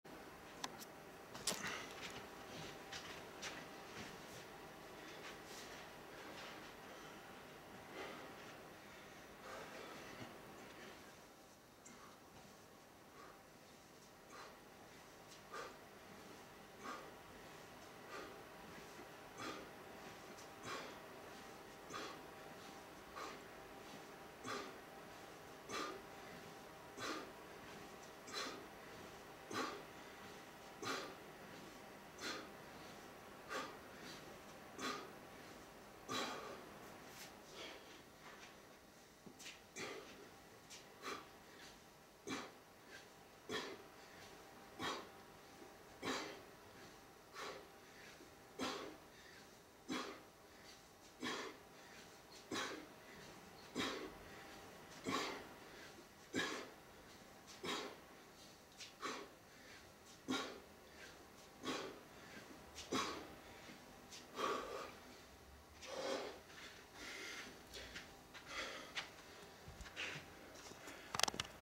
【28.2.5】ケトルベル的トレーニング。鉄アレイ7キログラムを使用。
お疲れ様です。マッスルサトーです。ケトルベルの代わりに鉄アレイを使用。ということで、ケトルベル的トレーニング！おそれいります。よろしければご覧くださいませ。